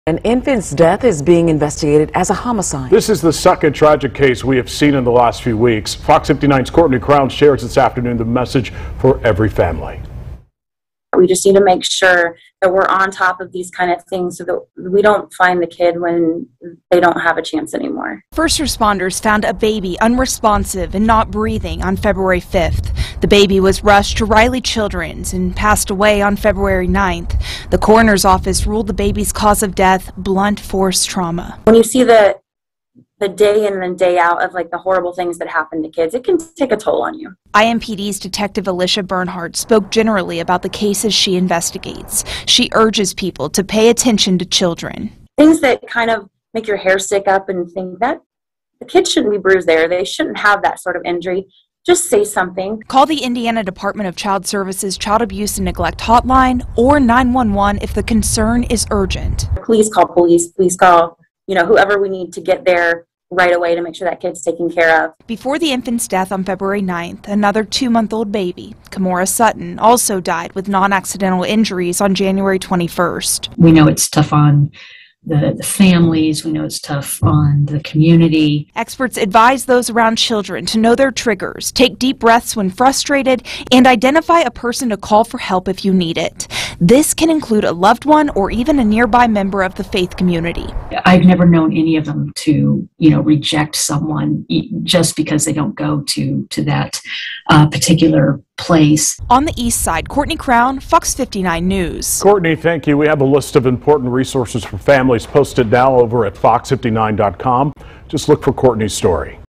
2-month-old's death ruled homicide by Marion Co. coroner
The Marion County coroner's office has declared the death of a 2-month-old child as a homicide. Meanwhile experts advise people on the best ways to prevent child abuse.